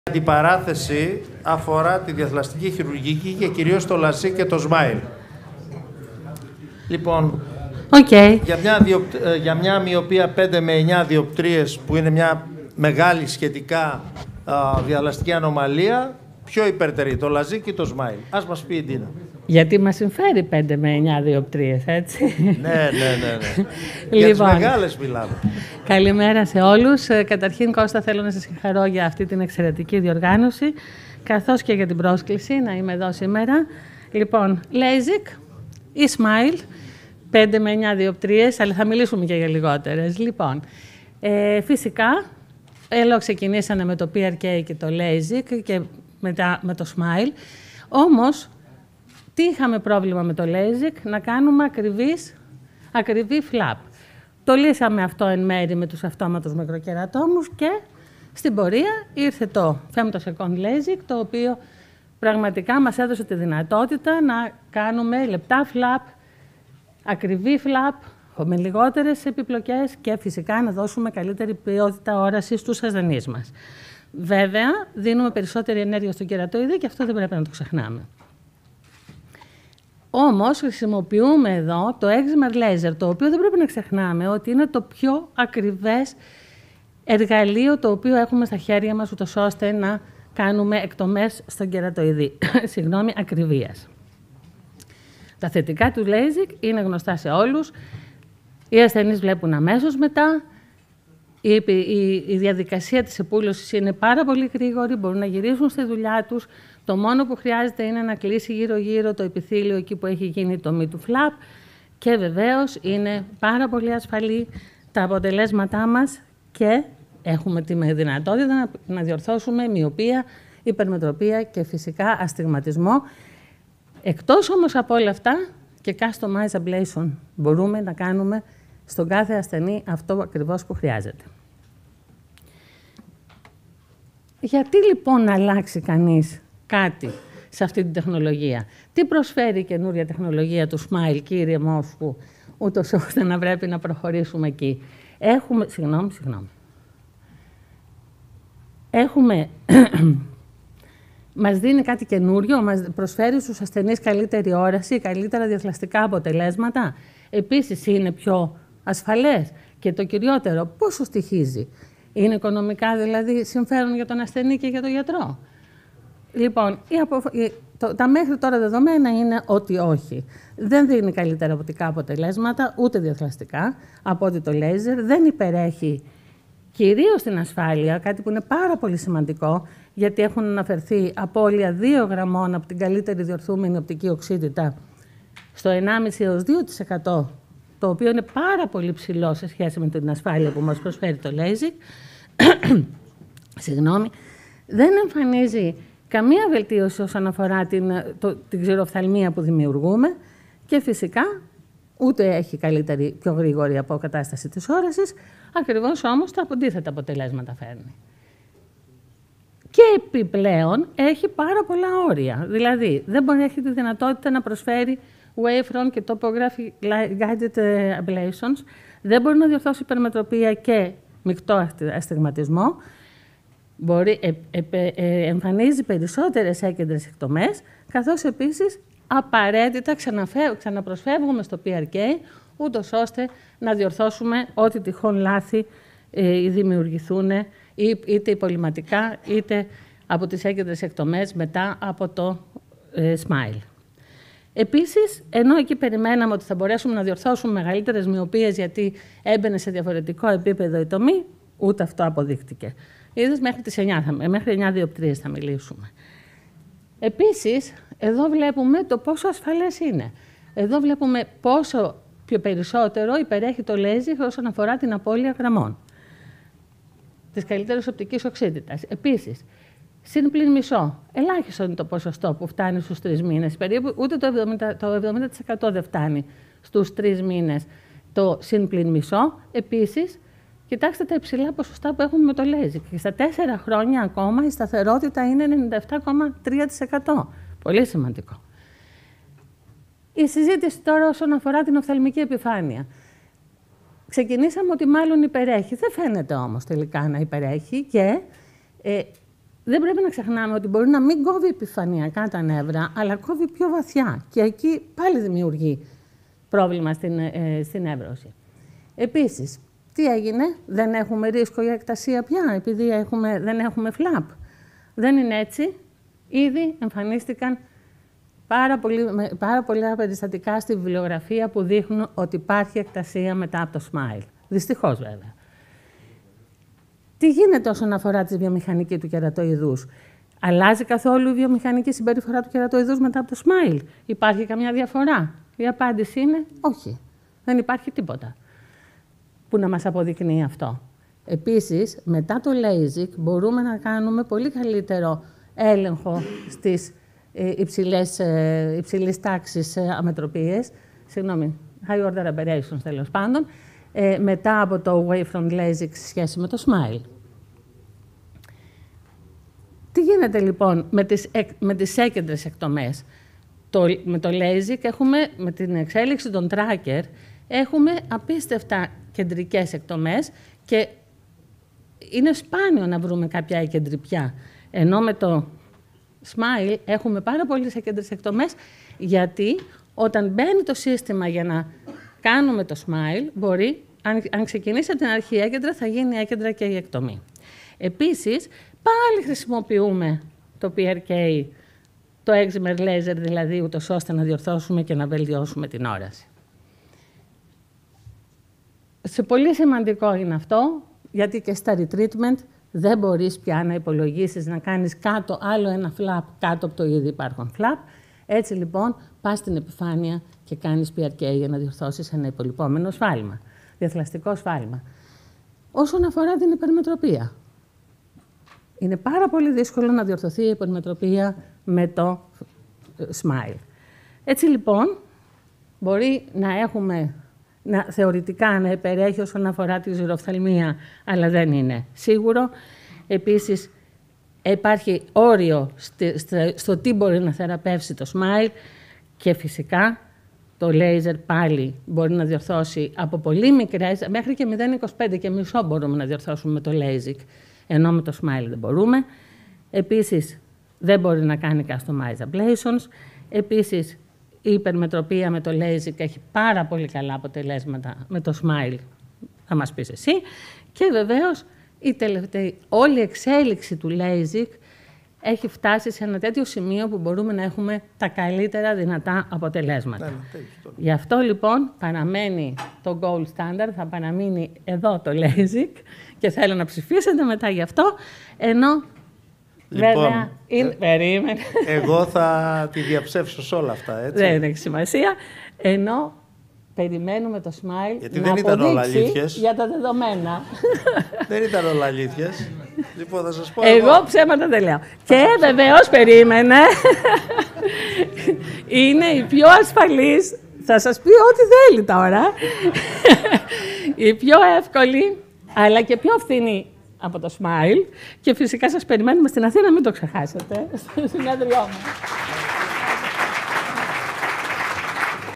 Κ. Κουφαλά
Αντιπαραθέσεις στη διαθλαστική χειρουργική
Προεδρείο: Κ. Κουφαλά
Αντιπαράθεση 1η:
«To LASIK σαφώς υπερτερεί απέναντι στο SMILE για τις μυωπίες 5-9 dpt.»
NAI
Αντιπαραθέσεις στην Οφθαλμολογία 2018